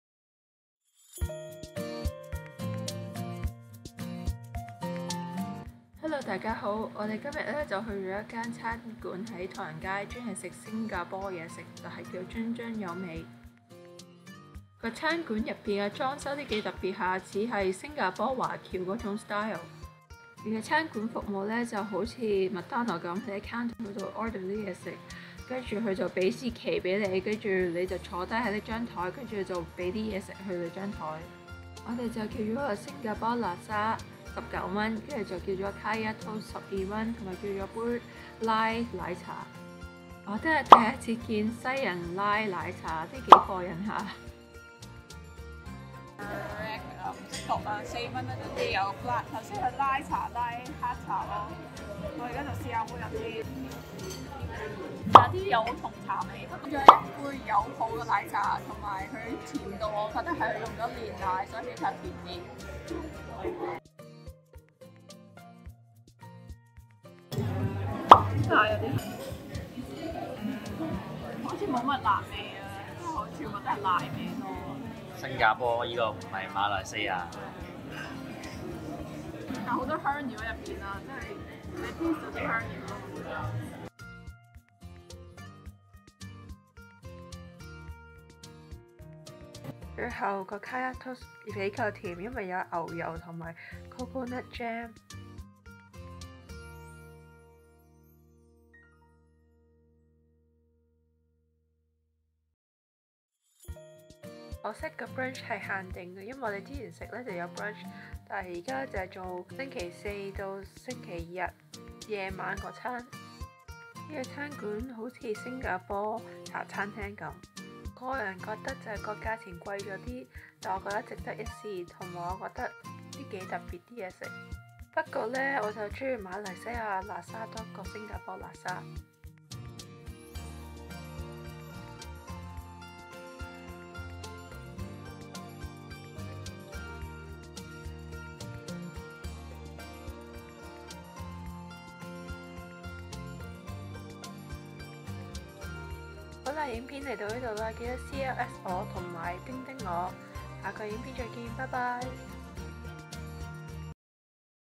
【溫哥華美食】星加坡嘢 叫津津有味| Singapore brunch in Chinatown? | Vancouver Foodie
Hello Everyone,
Welcome to NaNaOrange Channel! Check out this restaurant Nancy GoYaya in Chinatown! Nancy Go Yaya feature Singapore brunch before such as Kaya Toast, Singapore Laksa and their special milk tea. It felt like being transported to Singapore cafe.  If you love coconut milk flavour and the taste of sweetness this is your cup of tea! However, I realize they don't do Singapore brunch anymore but they have feature japanese food street snack which is coming on September 27 and have collaboration with tofino brewery. They do serve dinner from Thursday to Sunday. There are more hidden gem or restaurant in Chinatown, but I prefer to go in the morning or in afternoon unless I am in a big crowd.

Nancy GoYaYa Eating House
265 Pender Street E, VANCOUVER

The price is on reasonable side but it is worth trying!